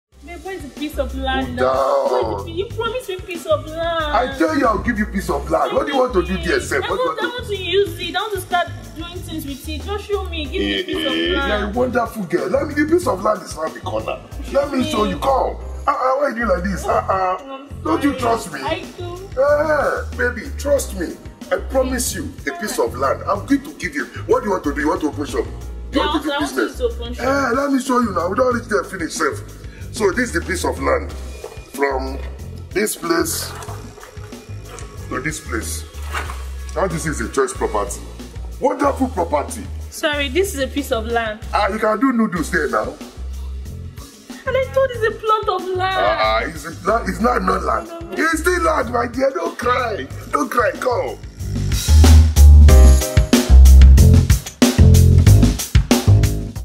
WATCH: Wale Scams His New Girlfriend
After his break-up with Amanda, Walepowpowpow now has a new girlfriend. But as he often does, he promised her something he couldn't afford.

What would you do if you were her?